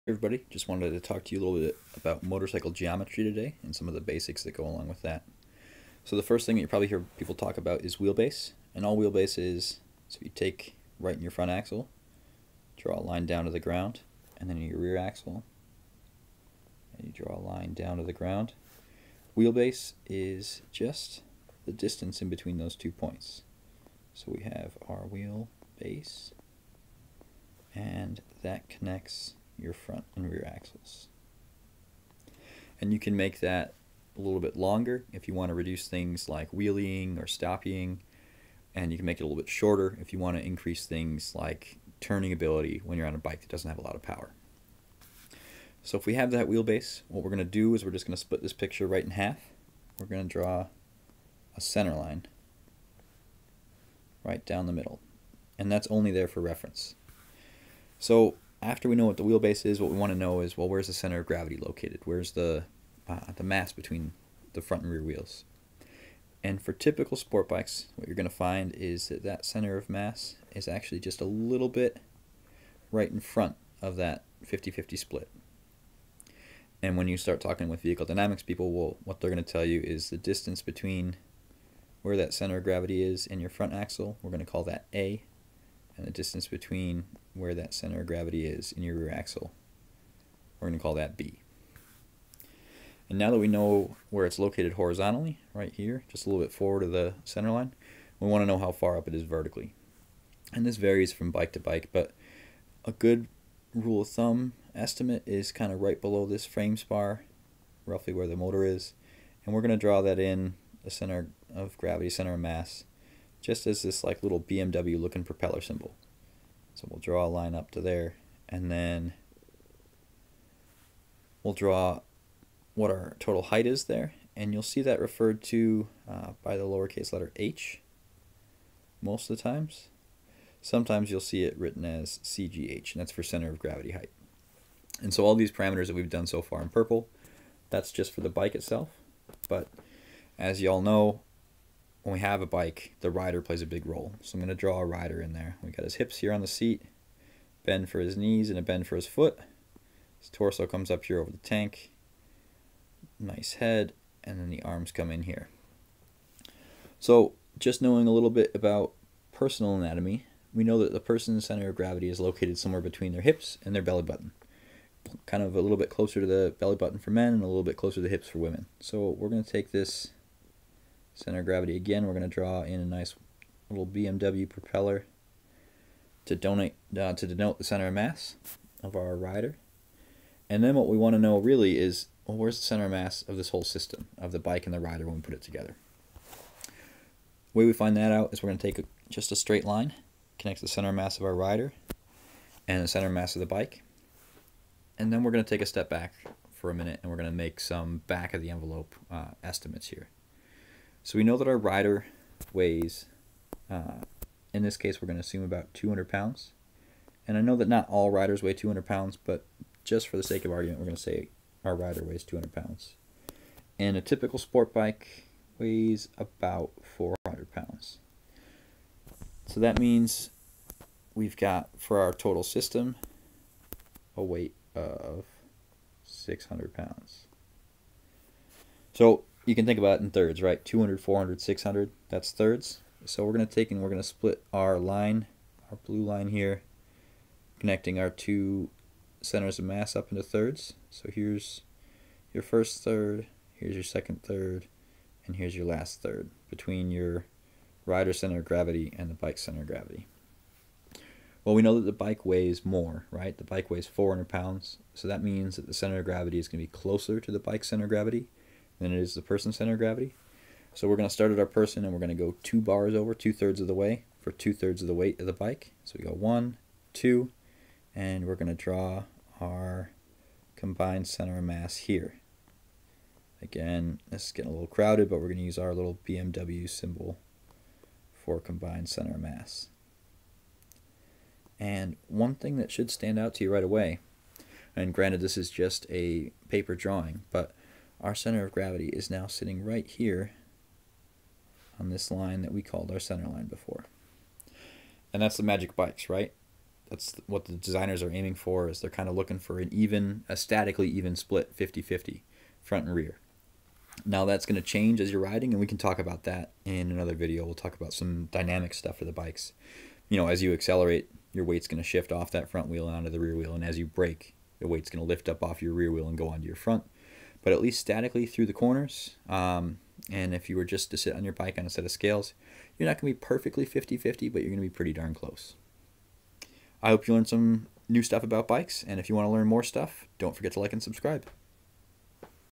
Basics of Motorcycle Geometry (Center of Gravity)
A brief guide to motorcycle geometry, talking about the center of gravity and where that fits in with the bikes wheelbase, with and without a rider.